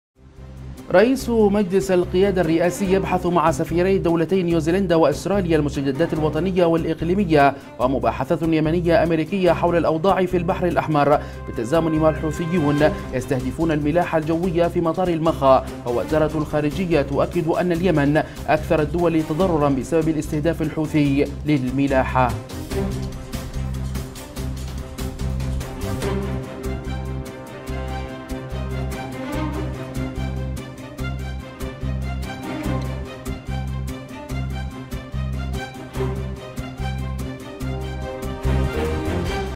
مقدمة المحور_السياسي
رئيس مجلس القيادة الرئاسي يبحث مع سفيري دولتي نيوزيلندا واستراليا المستجدات الوطنية والاقليمية ومباحثات يمنية أمريكية حول الأوضاع في البحر الأحمر وبالتزامن الحوثيون يستهدفون الملاحة الجوية في مطار المخا ووزارة الخارجية تؤكد ان اليمن أكثر الدول تضرراً بسبب الاستهداف الحوثي للملاحة